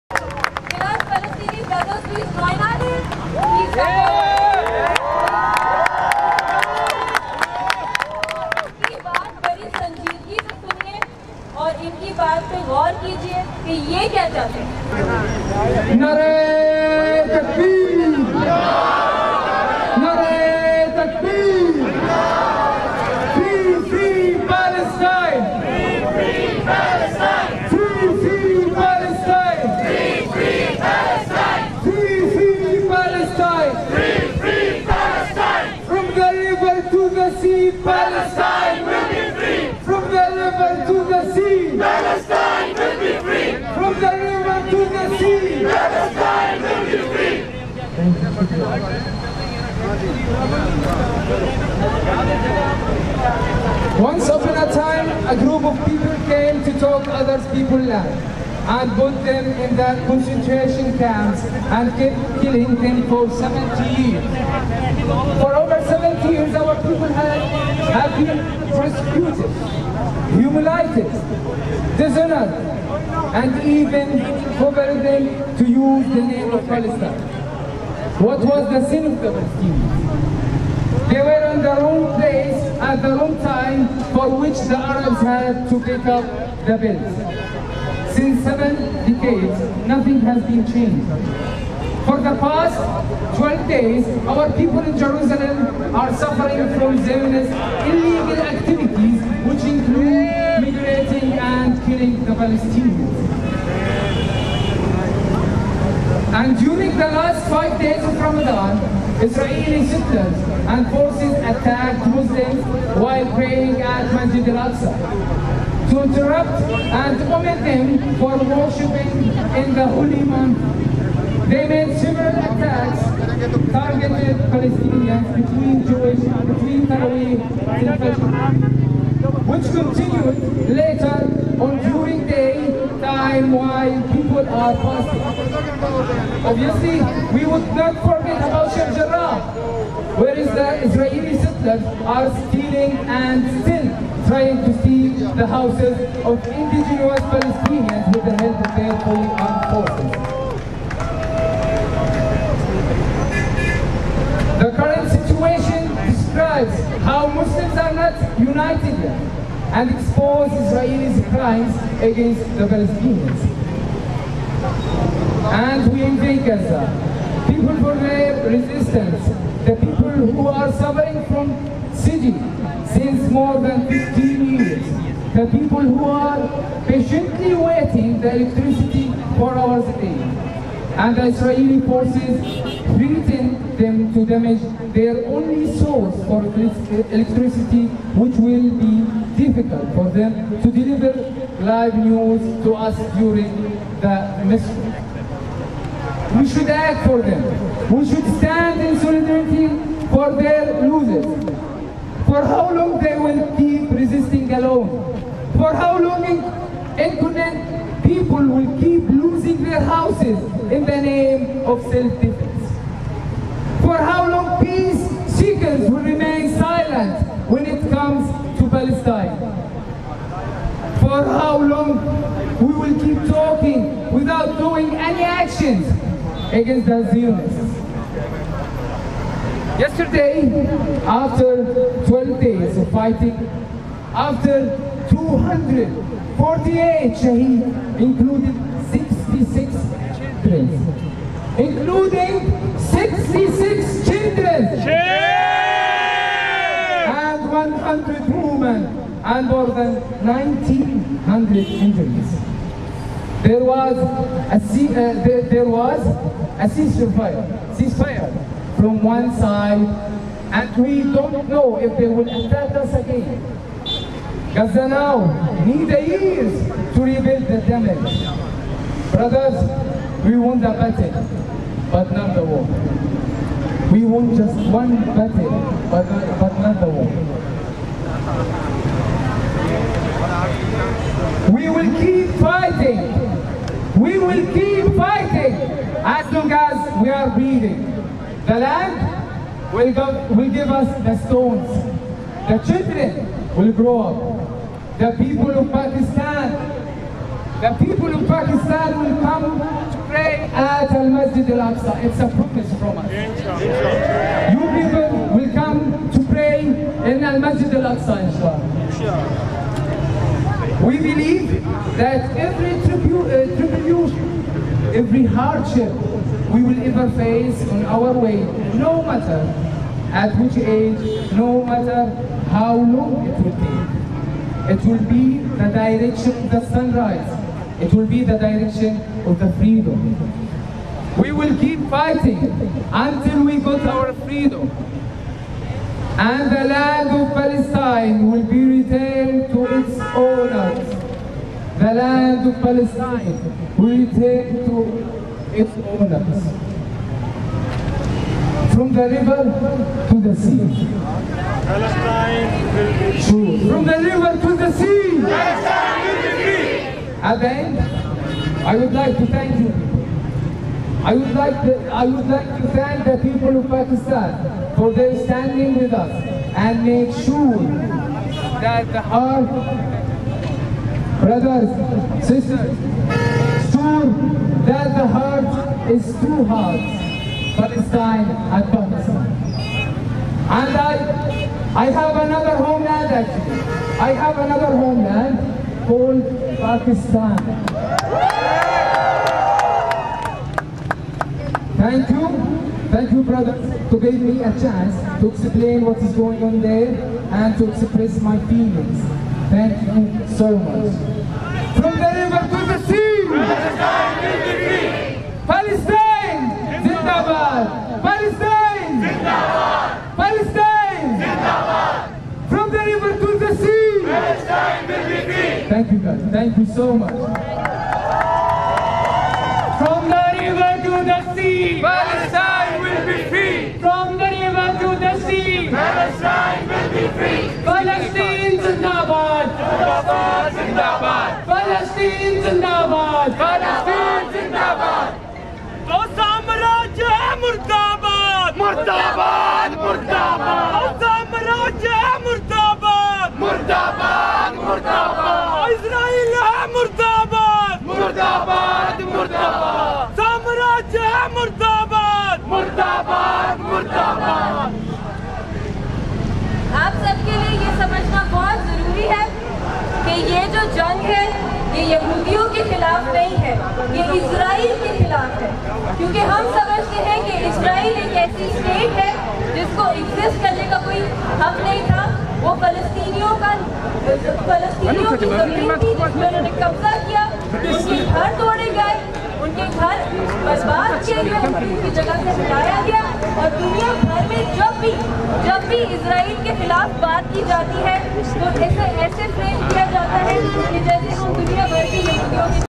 Palestinian Student Speech in Lahore | Lahore Peoples Welcome Palestine Students
شکریہ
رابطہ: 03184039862